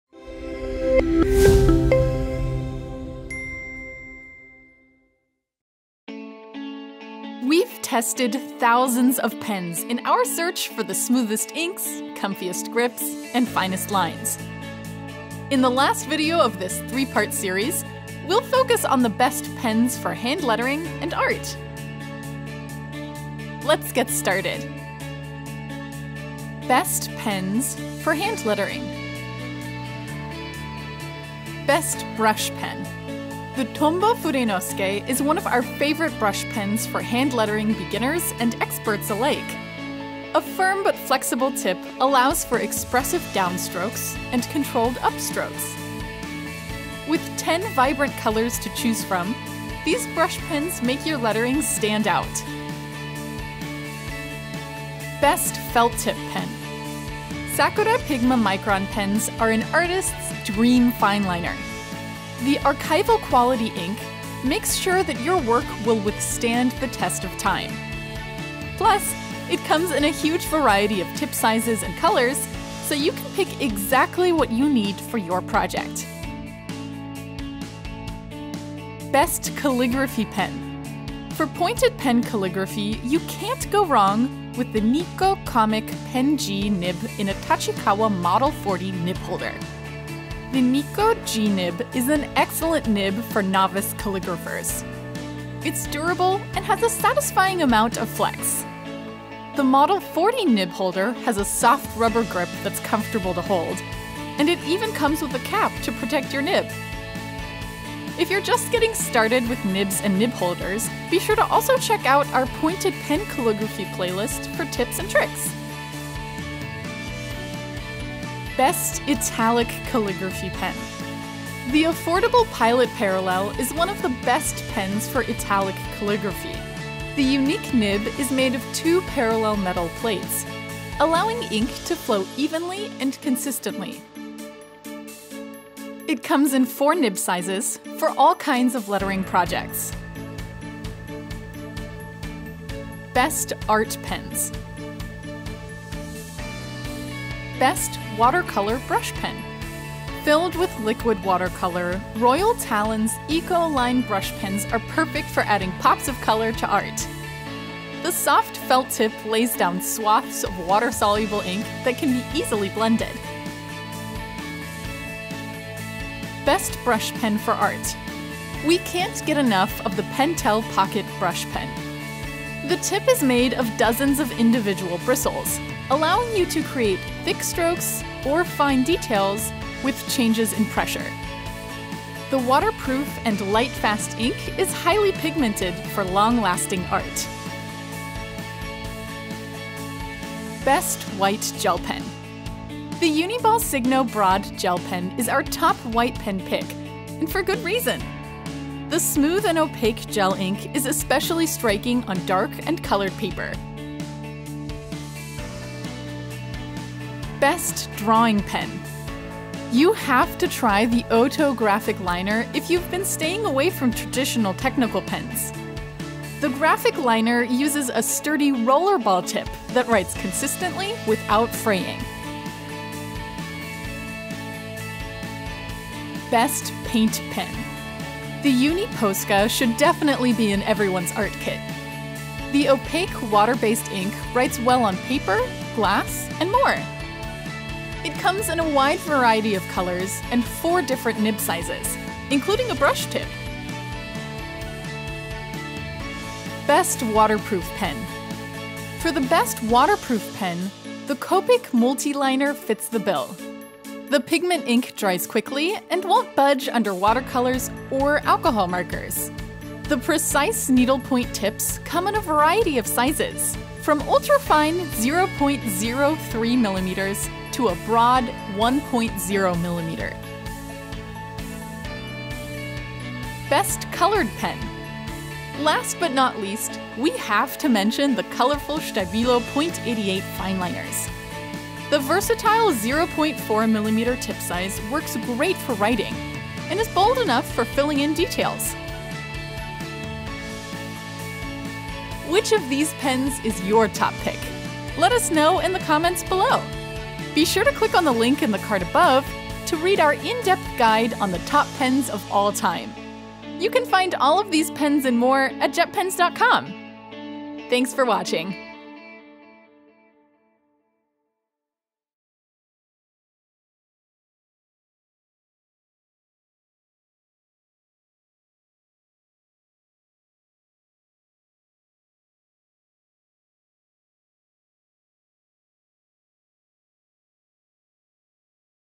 Top Pens For Hand Lettering & Art Of All Time
We share our top pens for hand lettering and art, including the Tombow Fudenosuke Brush Pen, Sakura Pigma Micron Pen, and Pentel Pocket Brush Pen. See if your favorites made our list!